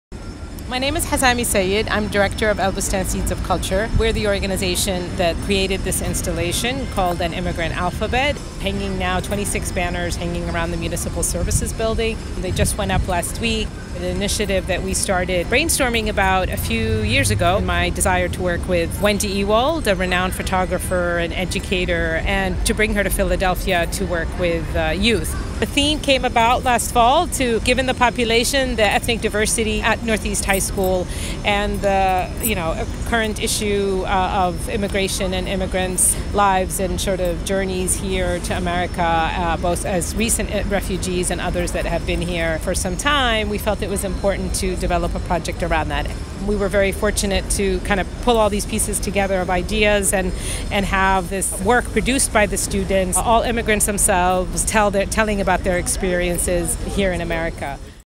"Immigration Alphabet"
Al-Bustan Seeds of Culture presents a collaborative project by artist Wendy Ewald and Northeast High School students with a series of free public events throughout the month of September.